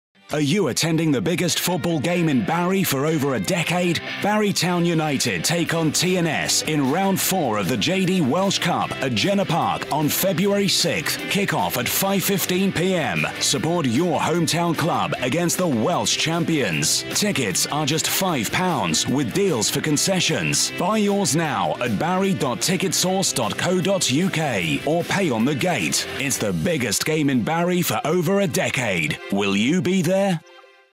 Barry vs The New Saints on Saturday 6 Feb at Jenner Park
Buy tickets for the Welsh Cup Fourth Round match at barry.ticketsource.co.uk or at the turnstiles on the day.